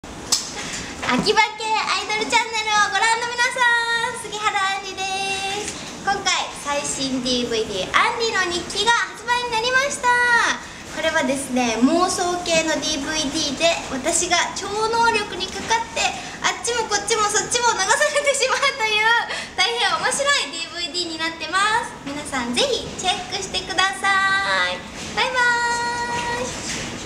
杉原杏璃動画コメント
『アンリの日記』DVD発売記念イベント。
DVDの内容は、超能力をかけられた杉原が次々と服を脱がされてしまうというストーリー仕立てになっており、演じてて笑っちゃったんですけど、じっくり観て私に超能力をかけた気分になって欲しいです。
お気に入りのシーンは、破ったTシャツ1枚でダンスを踊ってるんですが、激しく飛んだり跳ねたりしてるので見えるか見えないかがポイントです。
1番の見所は、体操のシーンでフラフープに挑戦したので腰の回し方をぜひ見て欲しいです。